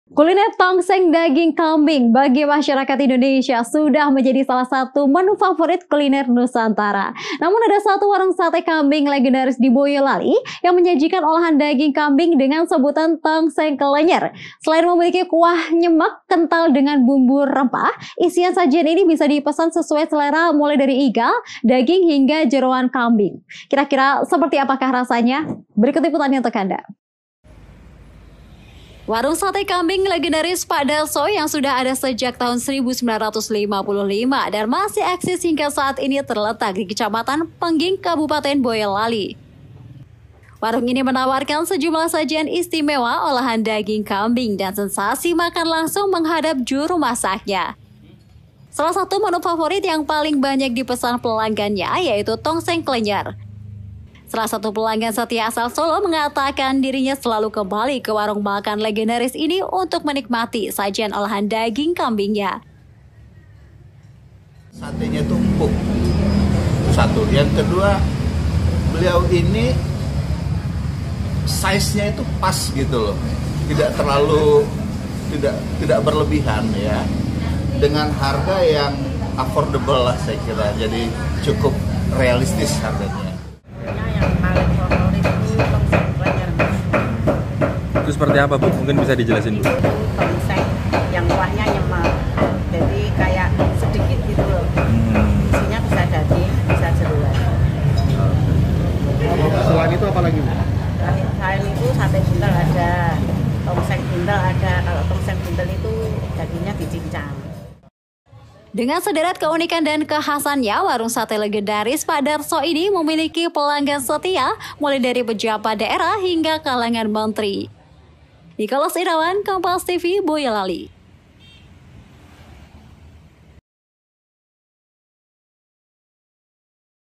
Warung Sate Kambing Legendaris Pak Darso di Boyolali Menawarkan Sajian Istimewa Daging Kambing Segar
PEKALONGAN, KOMPAS.TV - Inilah warung sate kambing legendaris Pak Darso yang sudah ada sejak tahun 1955 dan masih eksis hingga saat ini. Terletak di Kecamatan Pengging, Kabupaten Boyolali, Jawa Tengah, warung sate kambing legendaris ini selain menawarkan sejumlah sajian istimewa olahan daging kambing, juga menawarkan sensasi makan langsung menghadap juru masaknya. Daging kambing segar yang digantung di langit-langit atap dekat juru masaknya juga menjadi pemandangan khas dari warung sate legendaris ini. Daging segar itu baru akan dipotong-potong untuk dimasak setelah ada pesanan, sehingga para penikmat memang dimanjakan dengan daging kambing yang selalu segar.

Salah satu menu favorit yang paling banyak dipesan pelanggannya yaitu tongseng klenyer. Sajian ini tidak jauh beda dengan tongseng kambing pada umumnya, hanya saja olahan daging kambing yang satu ini dapat dipesan sesuai keinginan pelanggan untuk isiannya, mulai dari daging, iga, bahkan jeroan kambing. Selain itu yang menjadi khas dan berbeda dari tongseng lainnya yaitu kuah kental nan nyemek yang sarat bumbu rempah.

Dengan sederet keunikan dan kekhasannya, maka tak heran jika warung sate legendaris Pak Darso ini memiliki pelanggan setia mulai dari pejabat daerah, publik figur, pengusaha hingga kalangan menteri. Salah satu pelanggan setia asal Solo, Syarif mengatakan dirinya selalu kembali ke warung makan legendaris ini untuk menikmati sajian olahan daging kambingnya. Karena menurutnya selain legendaris dan sudah teruji kelezatannya, daging yang disajikan dalam aneka pilihan masakan ini selalu empuk saat menyapa lidah penikmatnya.

Dalam satu hari biasa diluar hari libur, dirinya bisa menghabiskan satu ekor kambing yang disajikan mencapai 300-an porsi dan habis hanya dalam kurun waktu 6 jam tiap harinya. Namun jika hari libur, warungnya bisa menghabiskan 2 hingga 3 ekor kambing per hari.

Untuk menikmati satu porsi olahan sate kambing dan berbagai menu salah satunya tongseng klenyer hanya merogoh kocek 26 ribu rupiah. Sedangkan untuk sate buntel cukup merogoh kocek 28 ribu rupiah.